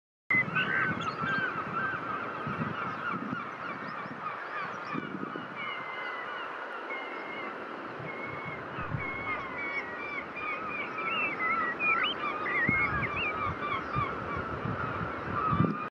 Aftermath of gull attack - mum preens chick
After a run in with gulls, mum stays close to her chick preening it's neck feathers. Filmed in June 2019.